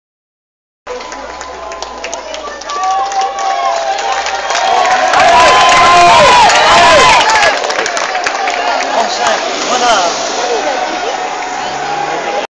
Tour de France 10 juillet 2013 à st Quentin Sur Le Homme -11ème étape
Contre la montre du Tour de France le 10 juillet 2013 à St Quentin Sur Le Homme :)